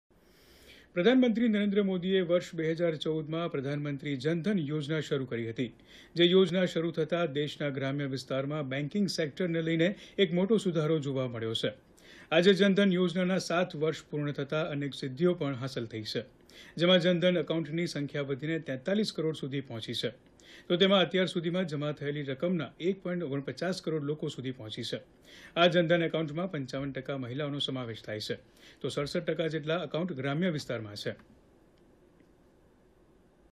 આજે જનધન યોજનાના 7 વર્ષ પૂર્ણ થતાં અનેક સિધ્ધીઓ થઇ હાંસલ
પ્રધાનમંત્રી નરેન્દ્ર મોદીએ વર્ષ 2014 માં "પ્રધાનમંત્રી જનધન યોજના" શરૂ કરી હતી. જે યોજના શરૂ થતાં દેશના ગ્રામ્ય વિસ્તારમાં બેન્કીંગ સેક્ટરને લઇને એક મોટો સુધારો જોવા મળ્યો છે. આજે જનધન યોજનાના 7 વર્ષ પૂર્ણ થતાં અનેક સિધ્ધીઓ પણ હાંસલ થઇ છે.